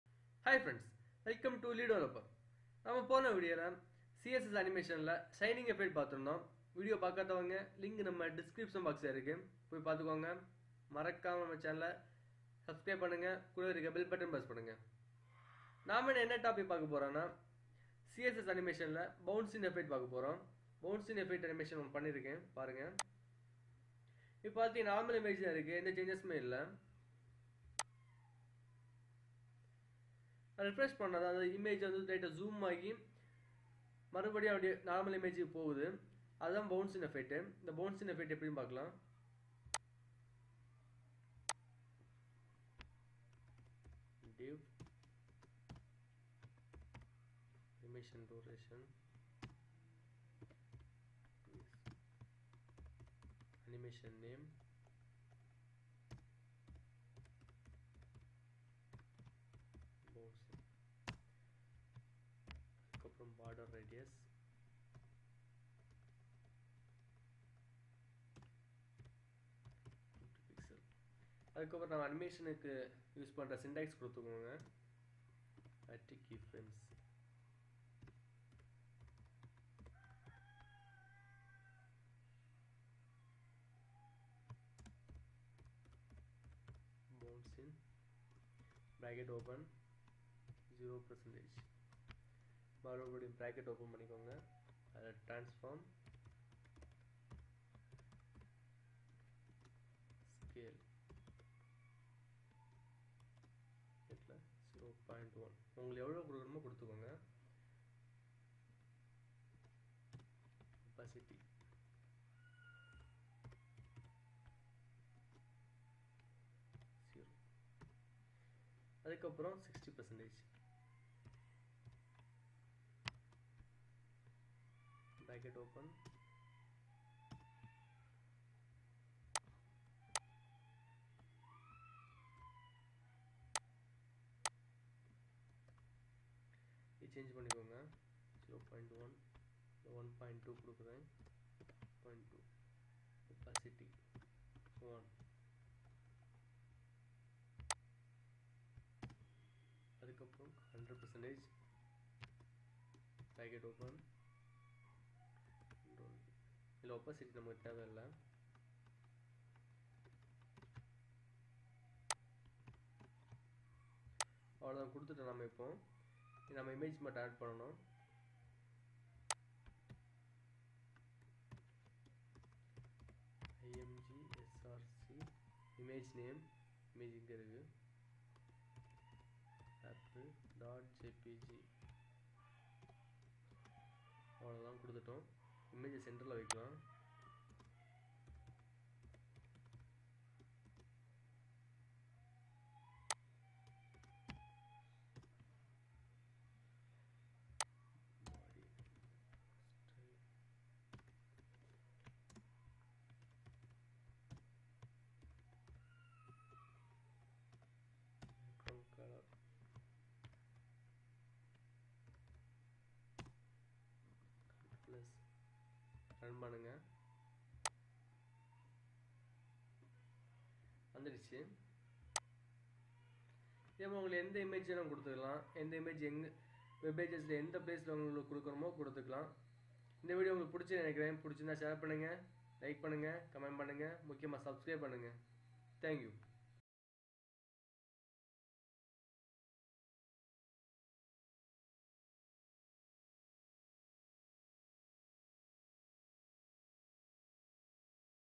create a bouncein text animation-css and html part17 webdesign tutorial in Tamilதமிழ்
webdesign complete tutorial for beginners in tamil

Watch "lee developer" on youtube